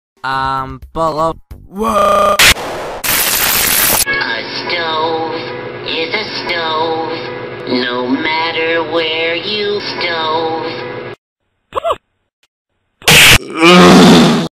a horrible excuse for a return to videopad tennises
Reupload from IceCreamJaxxie's old account, previously known as Lolman / The444thSpartan
Round 1 vs herself in her road to 100 rounds tennis
The difference between Carterkeown_ Alternate's reupload is that in some of parts I tried manually removing "round 1" text and AVS4YOU watermark where possible, since I couldn't find any parts of this round that didn't have AVS4YOU watermark or "round 1" text.

raymenchrom the new teegran gsargsyan!!11